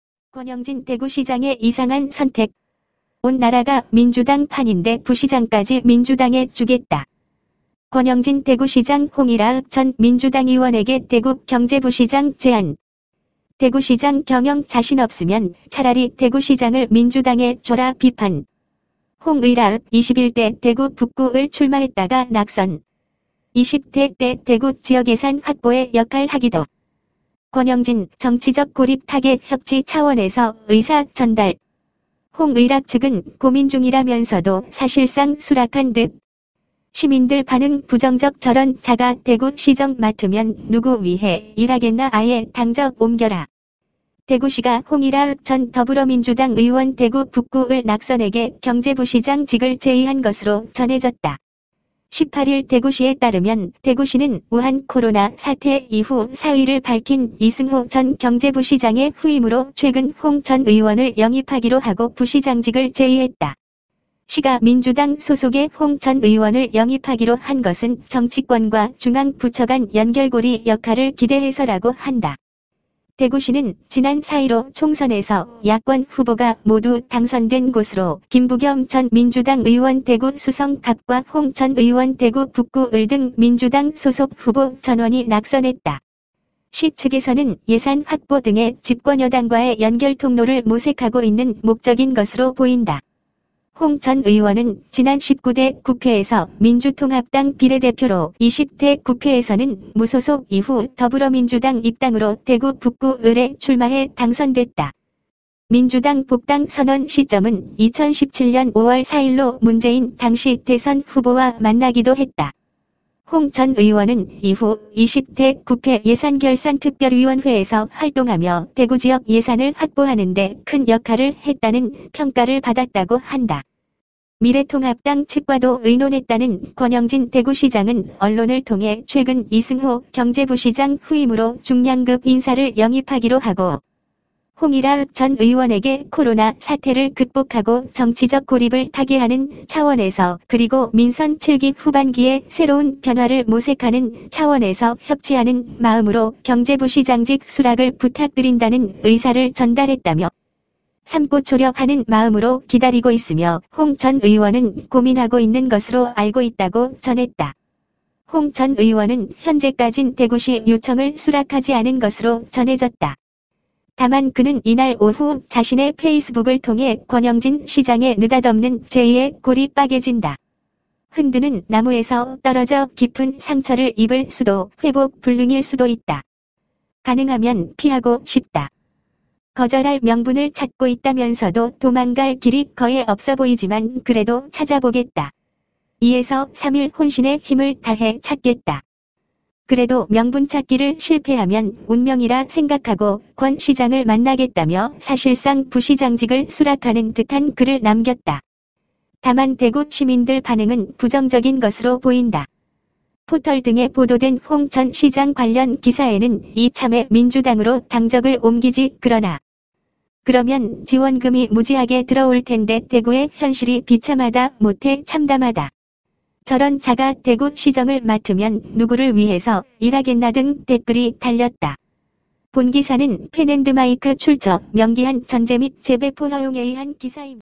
권영진 대구시장의 이상한 선택...온 나라가 민주당 판인데,부시장까지 민주당에 주겠다?
ⓒ 펜앤드마이크 출처 명기한 전재 및 재배포허용기사